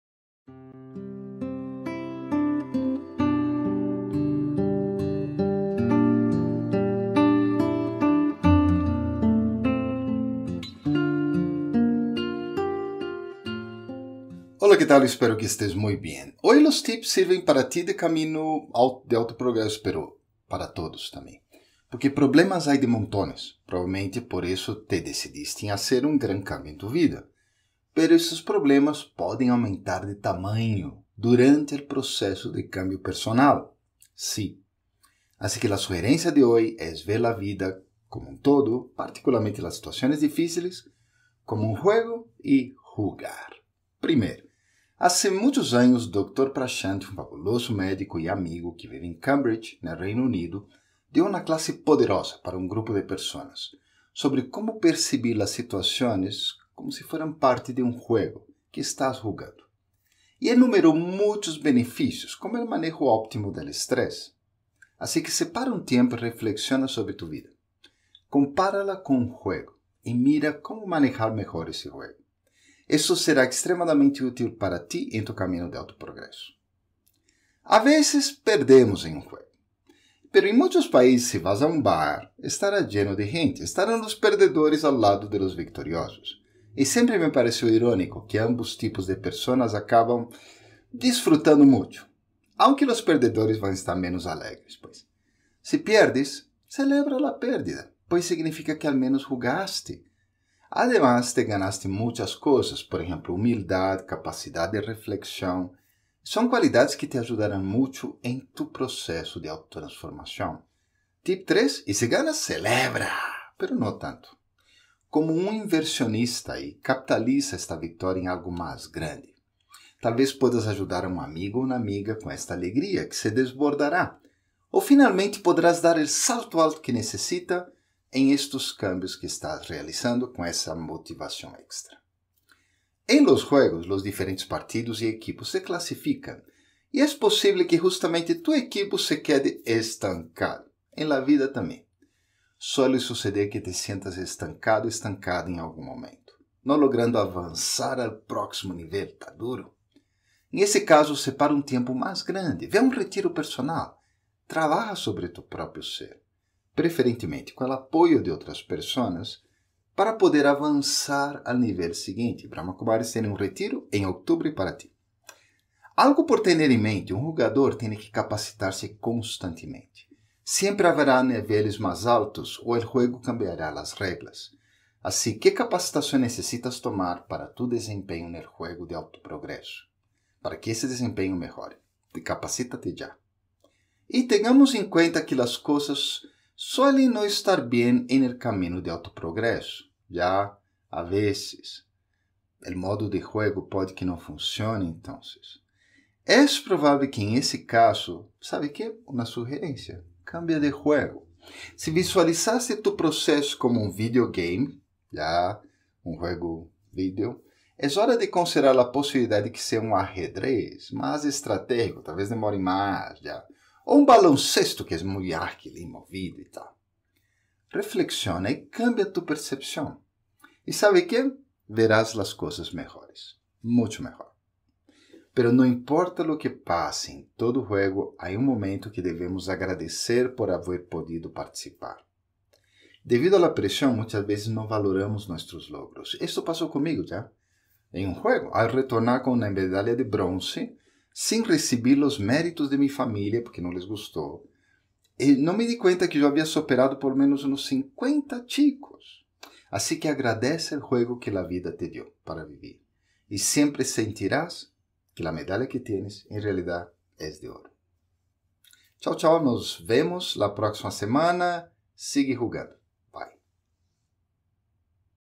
Tips para tu Autoprogreso - Porque la vida es un juego...
Problemas hay de montones, probablemente por eso te decidiste en hacer un gran cambio en tu vida. Pero estos problemas pueden aumentar de tamaño durante un proceso de cambio personal. Una sugerencia es ver la vida como un todo y particularmente las situaciones, como un juego. Y jugar...
1. Hace muchos años, el Dr. Prashant, un fabuloso médico y amigo que vive en Cambridge, Reino Unido, dio una clase poderosa para un grupo de personas sobre cómo percibir las situaciones como parte de un juego que estás jugando y enumeró muchos beneficios como el manejo óptimo del estrés. Separa un tiempo y reflexiona sobre tu vida, comparándola con un juego y mirando cómo manejarla mejor. Esto será extremadamente útil para ti en tu camino de autoprogreso.
2. A veces, perdemos en un juego... En muchos países, si vas a un bar, estará lleno de gente; estarán los perdedores y los victoriosos. Siempre me pareció irónico que ambos tipos de personas acaban disfrutando mucho, aunque los perdedores suelen estar menos alegres. Si pierdes, celebra la pérdida, pues significa que al menos jugaste. Además, te ganaste muchas cosas como humildad y capacidad de reflexión, cualidades que te apoyarán mucho en tu proceso de autotransformación.
3. Y si ganas, ¡CELEBRA! Pero no tanto; como un inversionista, capitaliza esta victoria en algo más grande tal vez puedas ayudar a un@ amig@ con esta alegría que se desbordará o finalmente podrás dar el salto alto que necesitas en estos cambios que estás realizando.
4. En los juegos, los diferentes partidos y equipos se clasifican y es posible que justamente TU EQUIPO se quede estancado; en la vida también, suele suceder que te sientas estancad@ en algún momento, no logrando avanzar al próximo nivel. En este caso, separa un tiempo más grande, ve a un retiro personal y trabaja sobre tu propio ser - preferentemente con apoyo de otras personas - para poder avanzar al nivel siguiente.
5. Algo por tener en mente: un jugador tiene que capacitarse constantemente. Siempre habrá niveles más altos o el juego cambiará de reglas; ¿qué capacitación necesitas tomar para que tu desempeño en el juego de autoprogreso mejore?
6. Tengamos en cuenta que si las cosas no salen bien en tu camino de cambio personal y el modo de juego no funciona, es probable que tengas que cambiar de juego. Si visualizaste tu proceso como un videogame, es hora de considerar la posibilidad de que sea un ajedrez - más estratégico - o baloncesto - muy ágil y movido. Reflexiona y cambia tu percepción, las cosas se harán mejores.
7. No importa lo que pase, en todo juego hay un momento que debemos agradecer por haber podido participar. Debido a la presión, muchas veces no valoramos nuestros logros - esto pasó conmigo, al retornar con una medalla de bronce, sin recibir los méritos de mi familia y no viendo que había superado unos 50 chicos por lo menos... AGRADECE el juego que la vida te dio para vivir y siempre sentirás que tienes la medalla de oro.